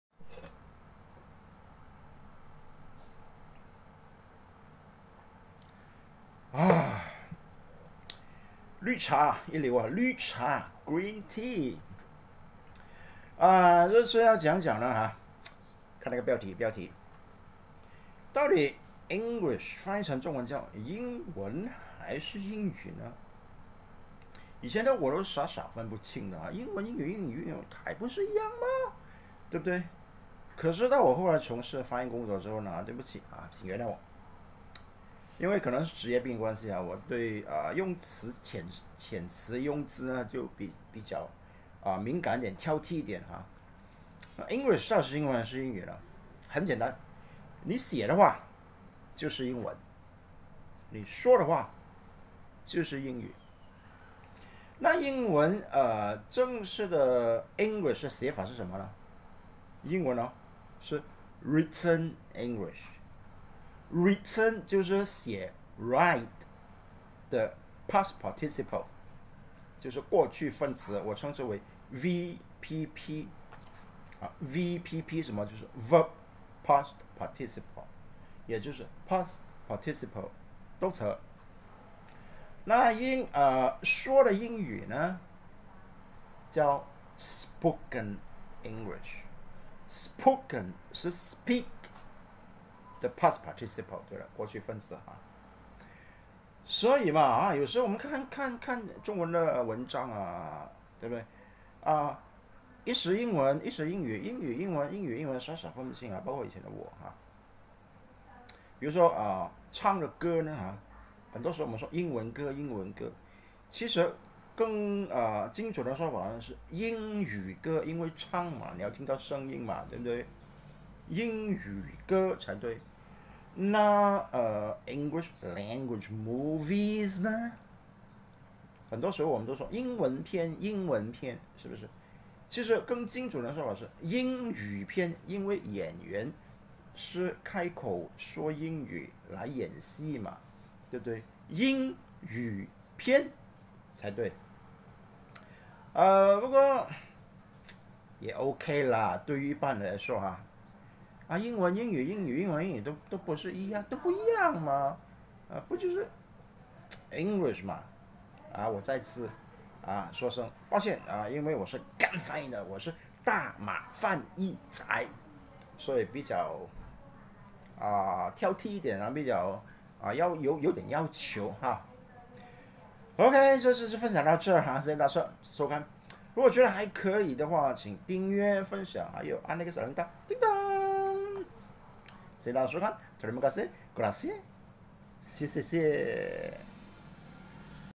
大马翻译仔：到底是英文还是英语？Spoken English？Written English？傻傻分不清
欢迎大家观看《全球华人学英语》其他视频，精彩可期，